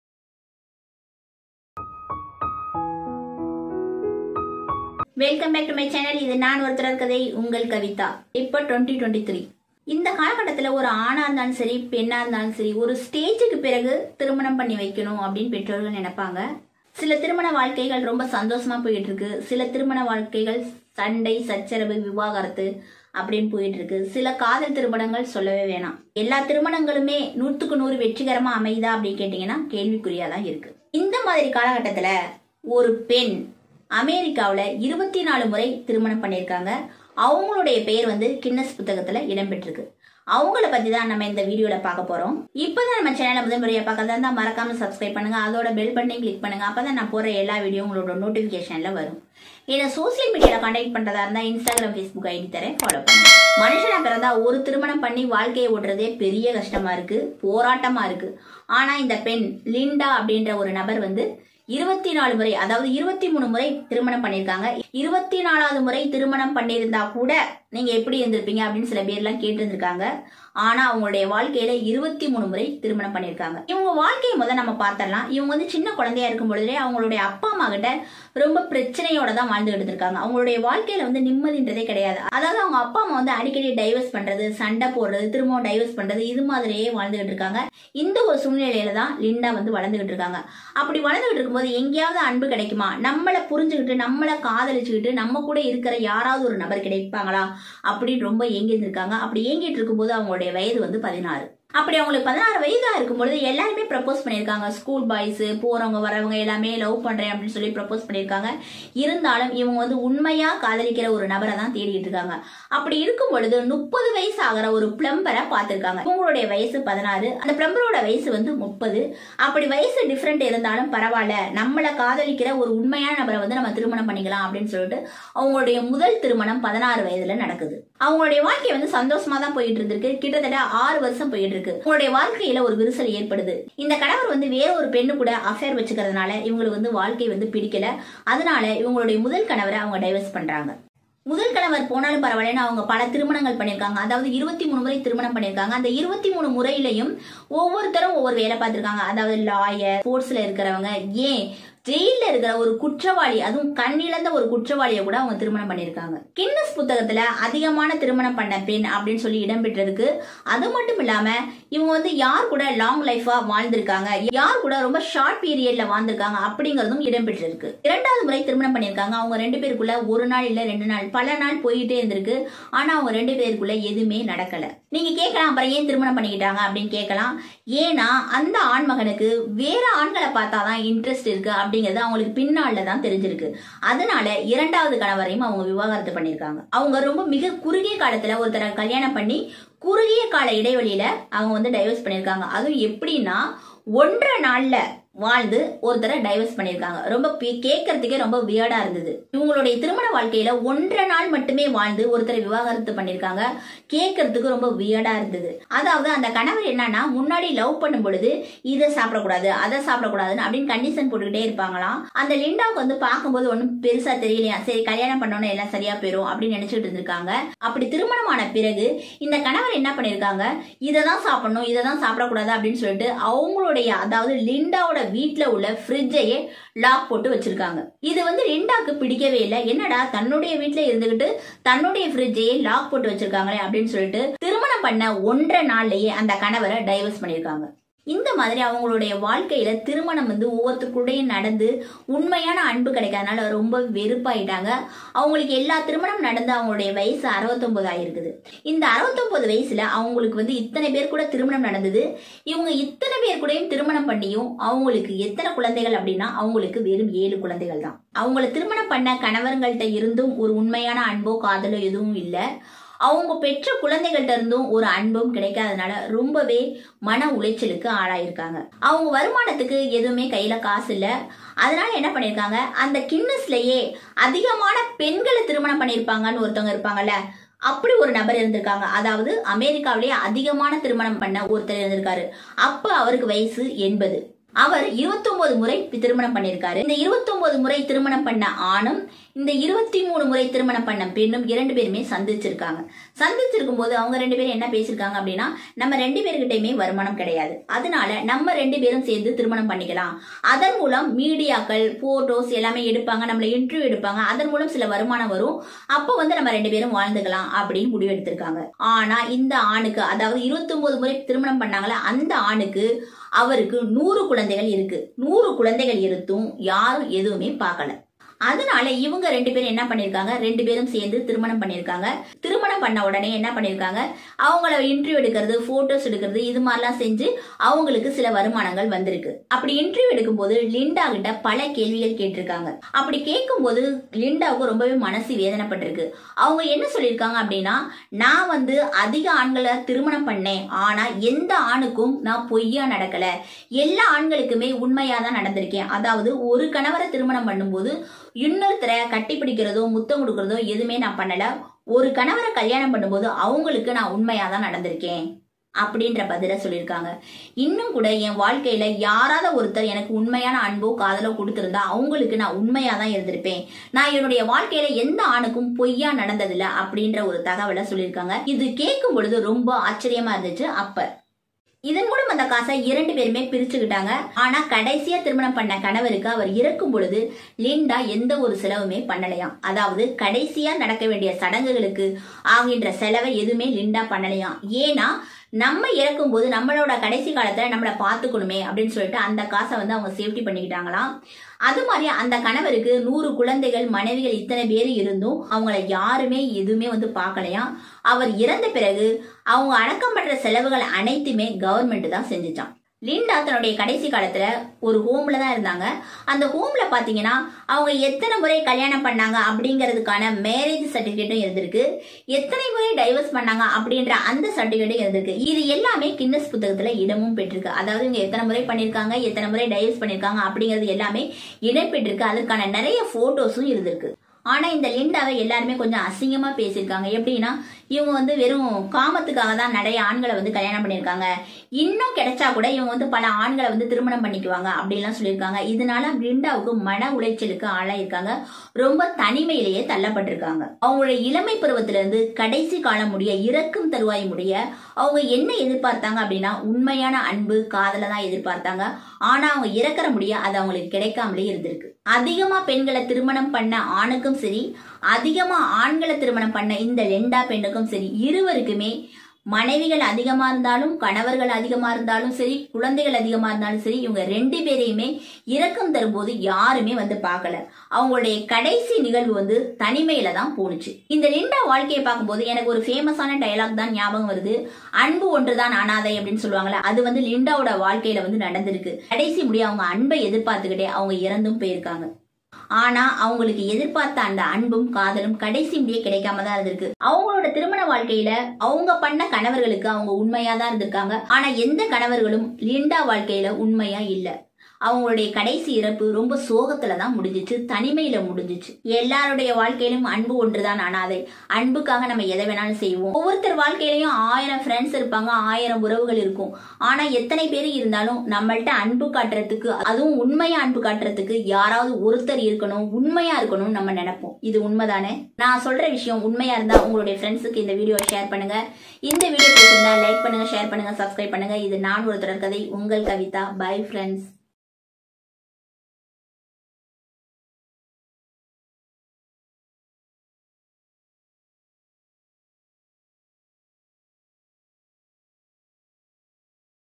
Linda wolf|Most married women explained tamil|23 times married husband|in tamil
In this video Explained The world's most married woman.